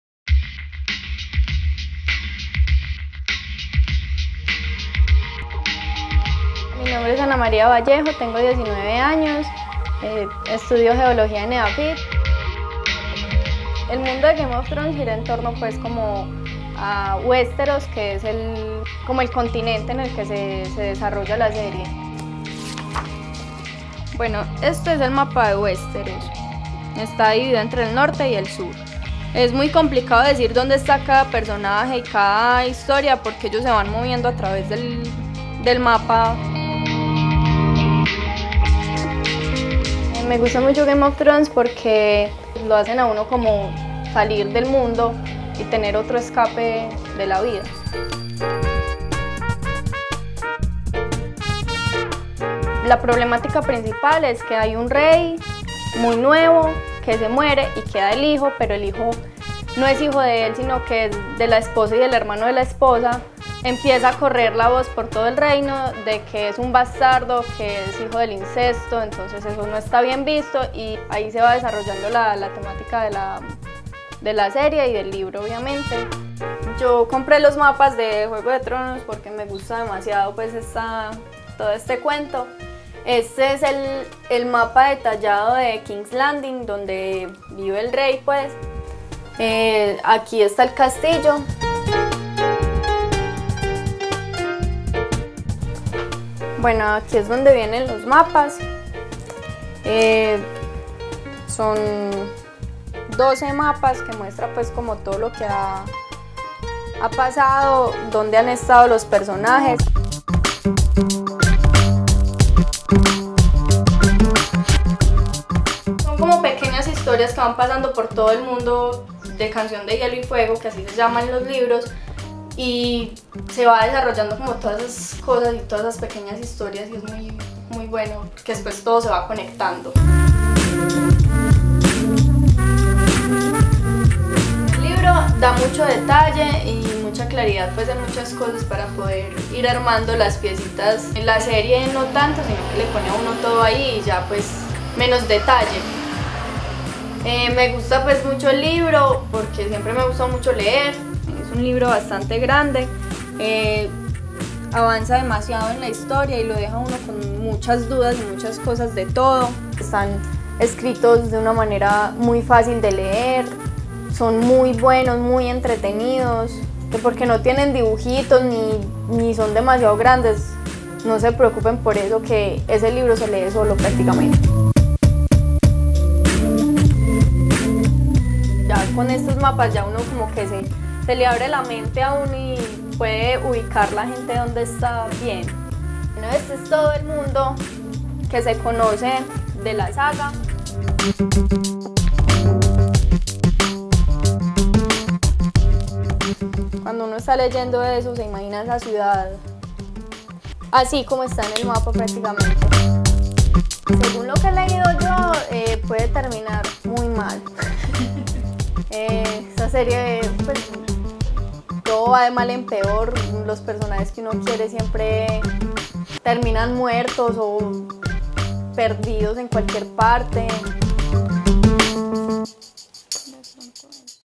Ana María Vallejo y los mapas de Game of thrones
Conocimos a una estudiante de geología de la universidad Eafit, fanática  de  los libros y la serie "Game of thrones", así que decimos invadirle el "rancho" y conocer más acerca de esta chica apasionada por los mundos fantásticos y medievales.